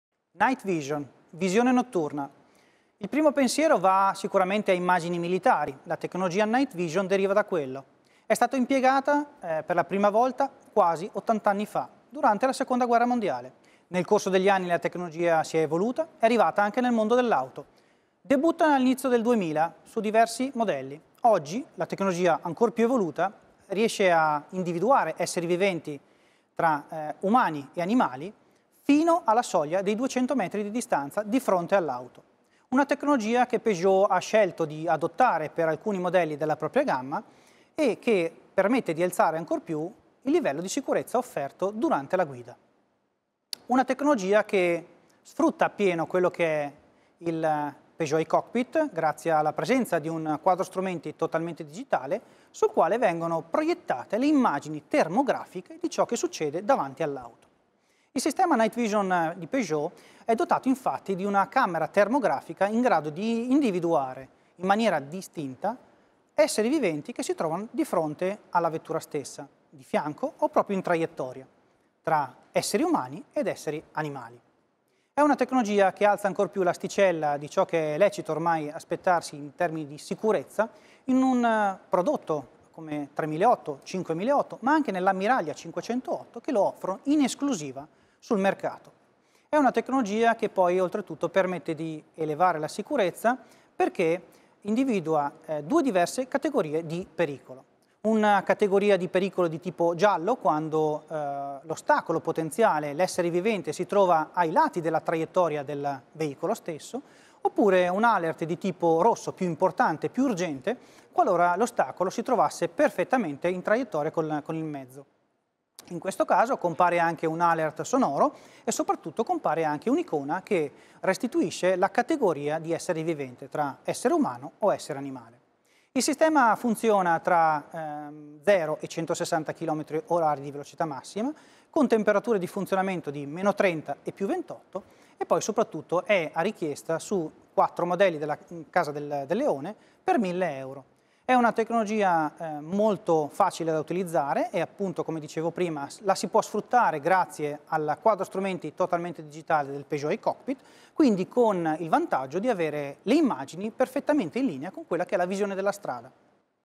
Occhio lungo di Peugeot in mezzo al buio della notte - Motor News n° 4 (2021)
Il sistema Night Vision di Peugeot permette di vedere ostacoli, animali o persone in mezzo alla notte, offrendo un grande contributo alla sicurezza. Nel video, Giulio Marc D’Alberton, Responsabile Comunicazione Peugeot Italia, illustra come si è sviluppato questo sistema e i particolari nell’applicazione in casa Peugeot.